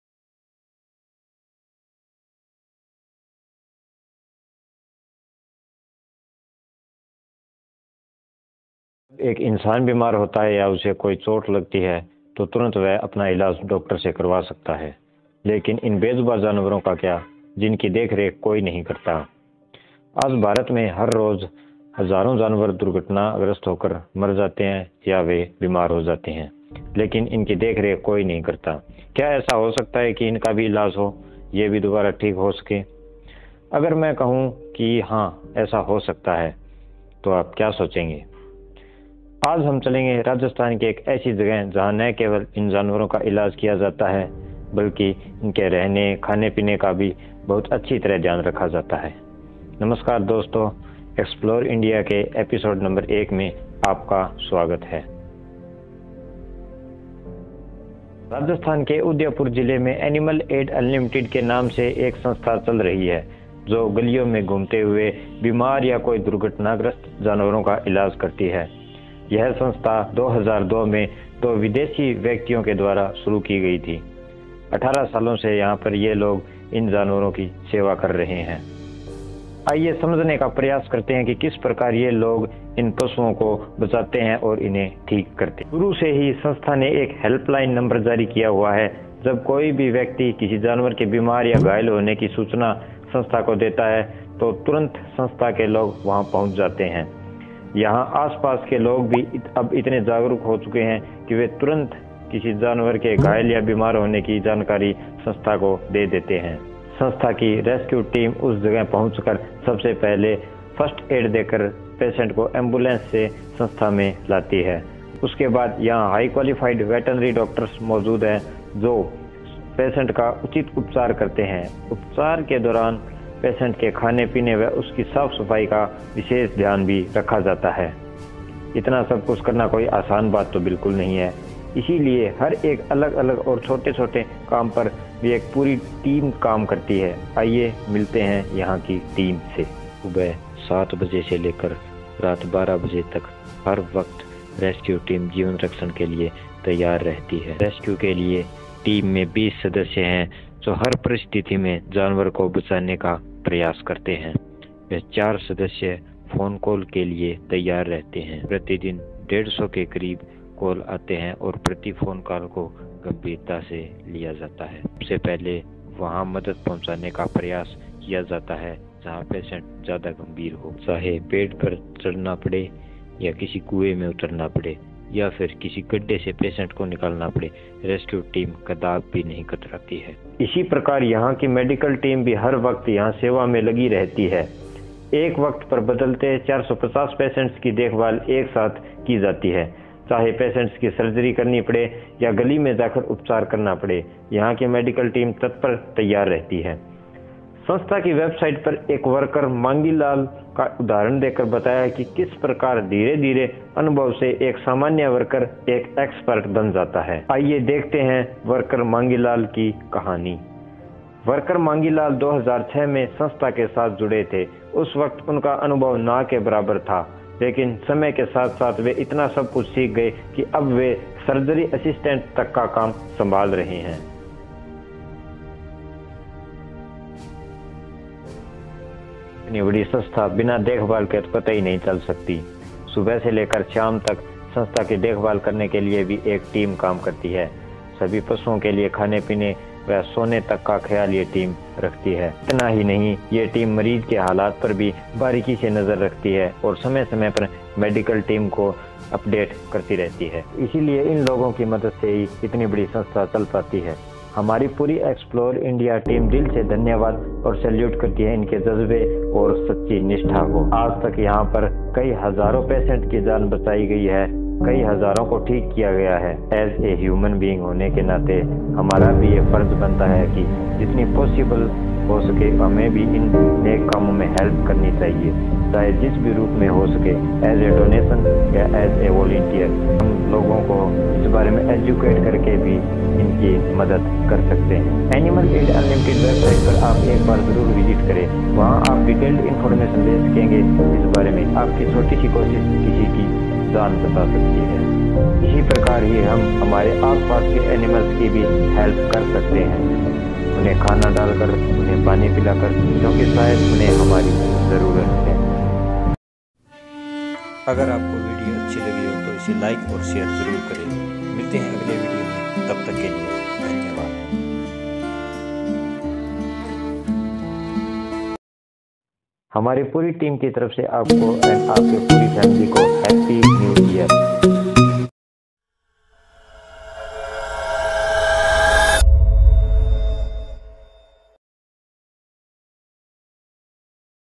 Aid Unlimited, India|| The Best Treatment Center ||Explore India
-Animal Aid Unlimited is an organization that treats injured and sick animals roaming in the street, in today's video we will talk in detail how these people save these animals and cure them. And know how different teams work behind all this.